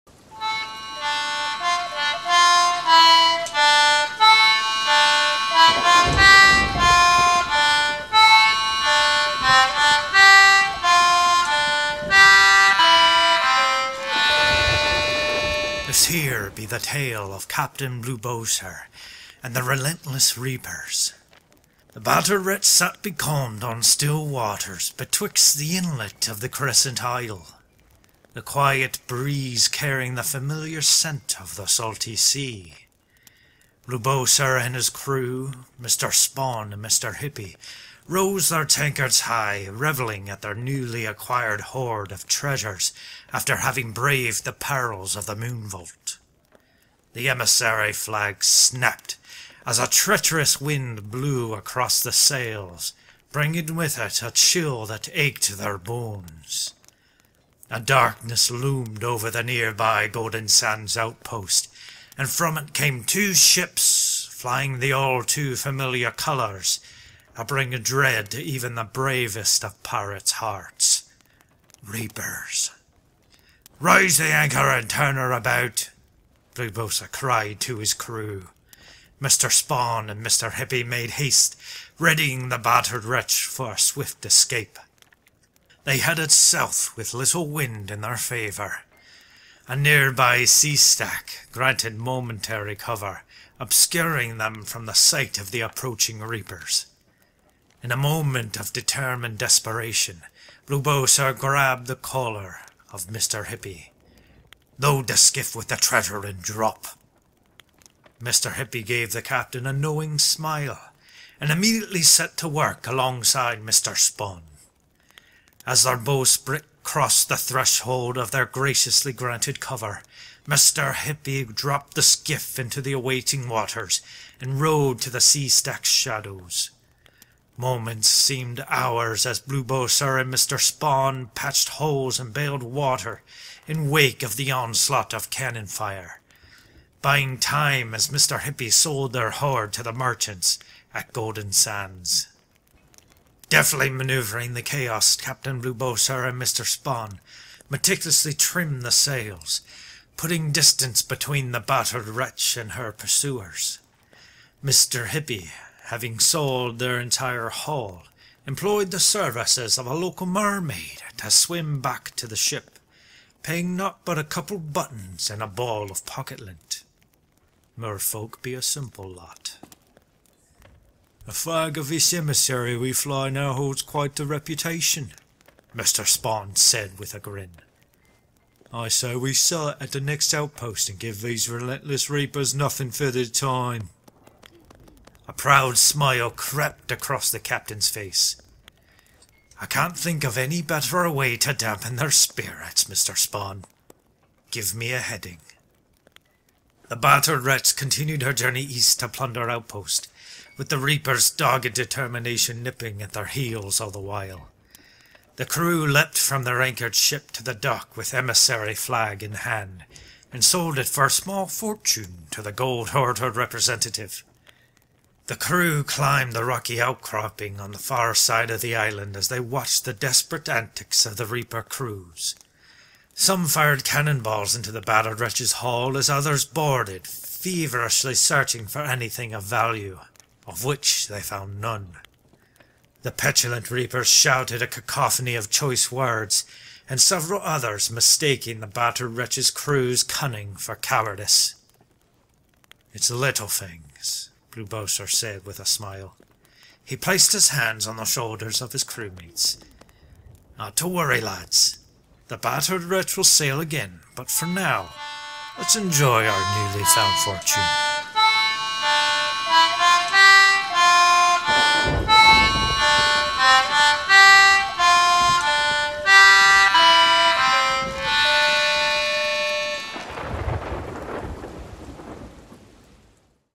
Captain Blubosa's Tavern Tales: The Relentless Reapers
Welcome to Captain Blubosa's Tavern Tales! Today's tale is that of an unfortunate run-in with the reapers and the cunning efforts of the Battered Wretch's crew.